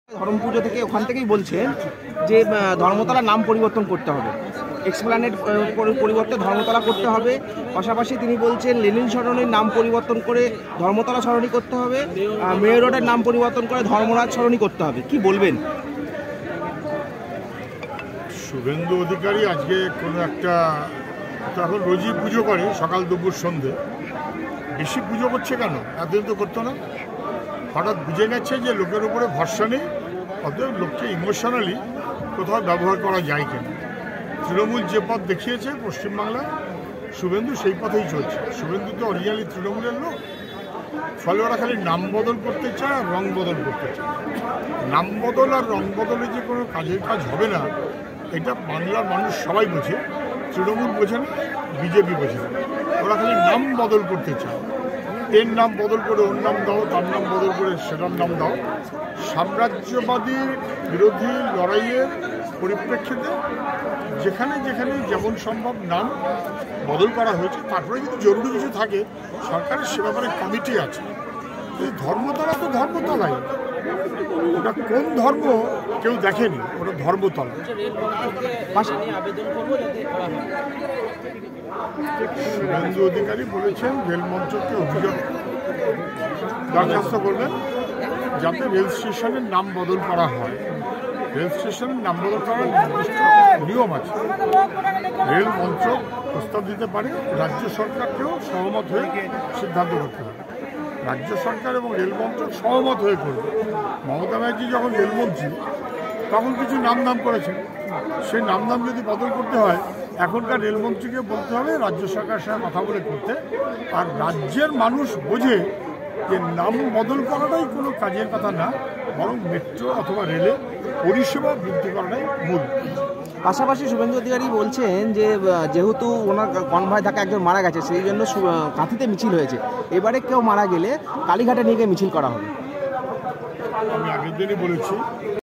হিম্মত থাকলে লেনিন সরণী বা এসপ্ল্যানেডের নাম পরিবর্তন করে দেখাক - পাড়ুইয়ে হুঙ্কার সুজন চক্রবর্তীর
|𝑨𝒃𝒐𝒖𝒕 𝑪𝑷𝑰(𝑴)|

|𝑲𝒆𝒚𝒘𝒐𝒓𝒅s|